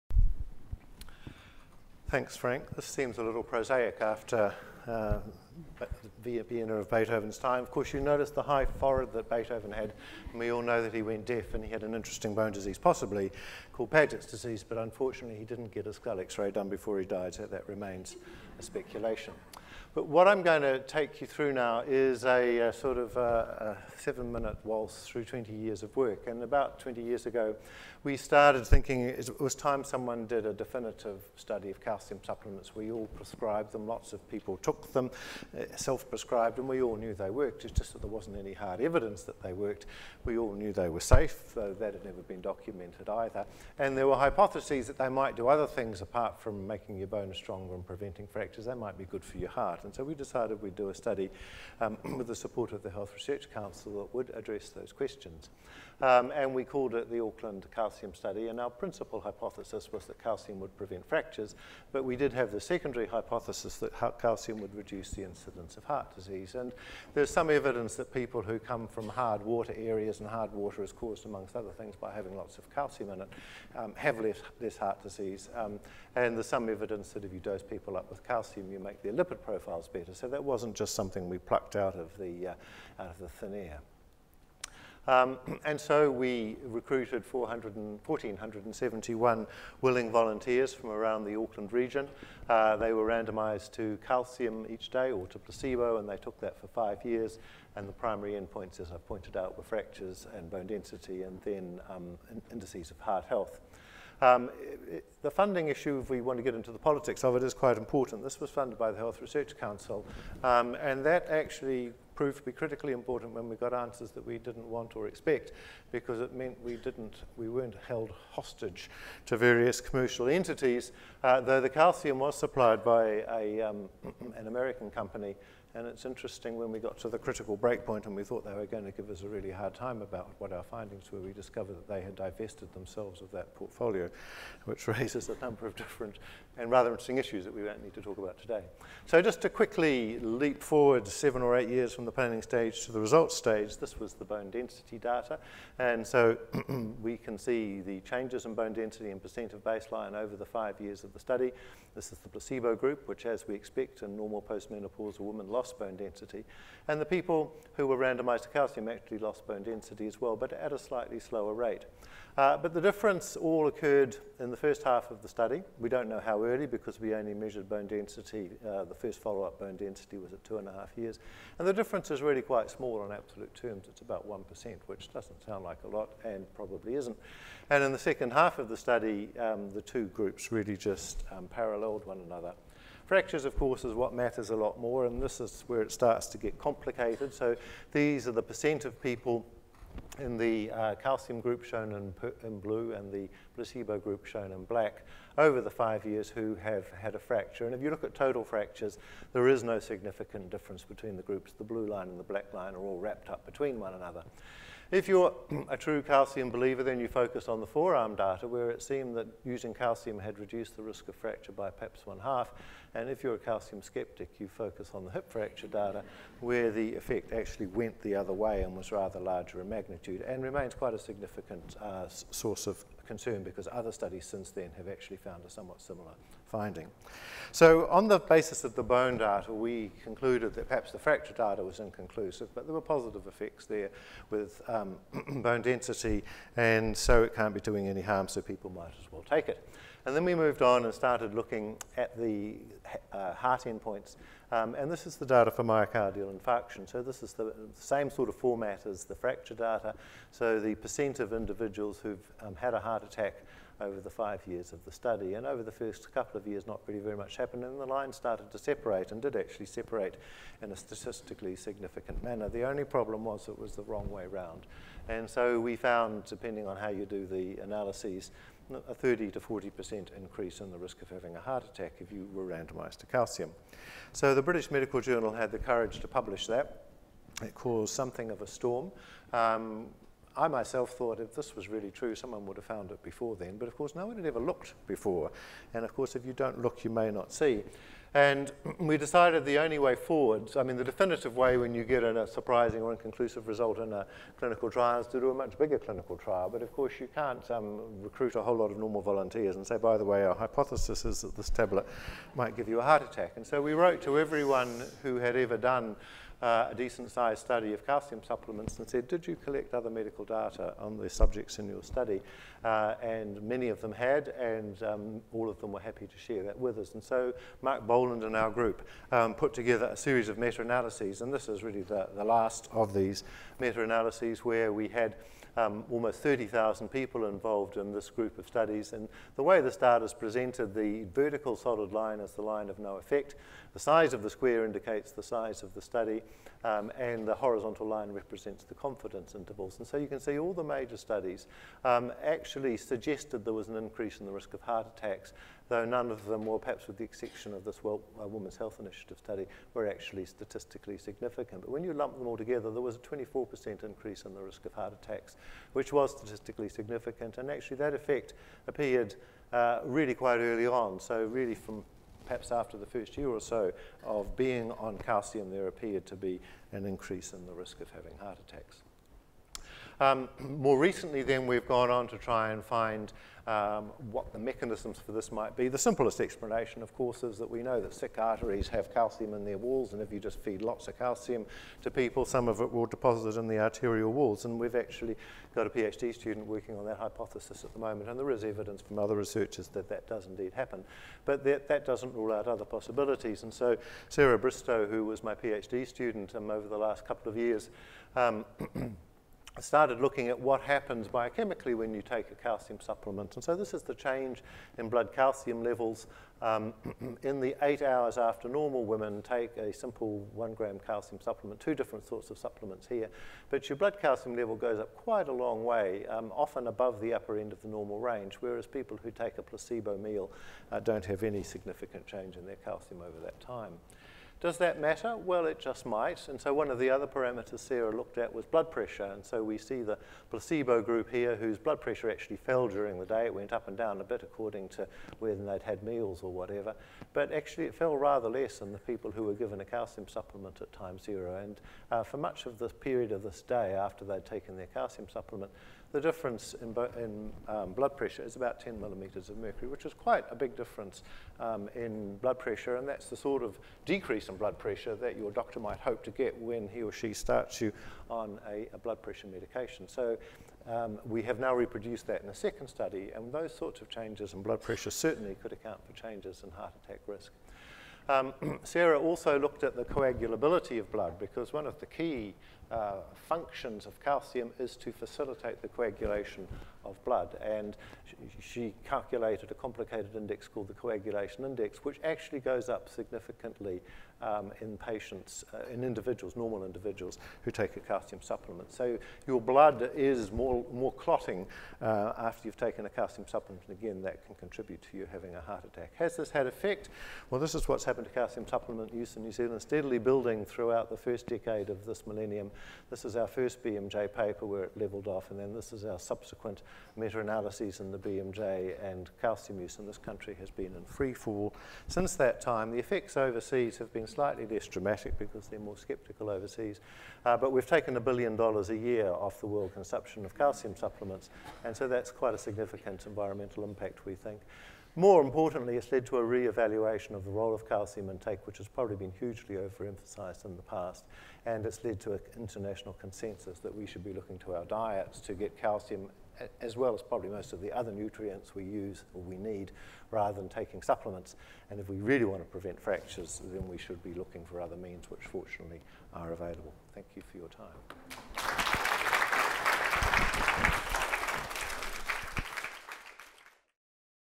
Unexpected side-effects from calcium supplements
Distinguished Professor Ian Reid presents the Unexpected side-effects from calcium supplements at the 2016 Research Excellence Forum.